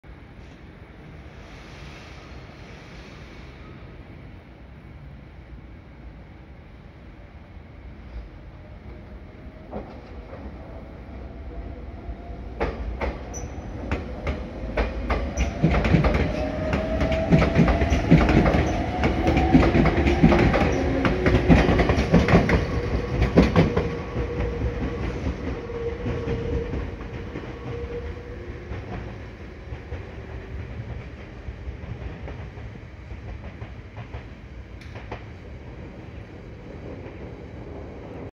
●20220125 3500系 発車 ＠東岡崎
撮影路線：名鉄 名古屋本線
撮影駅：東岡崎
行先：普通 岩倉
車両：3500系
両数：4両
撮影機材：【スマホ】SONY Xperia XZ2 Premium
MH：ーー
備考欄：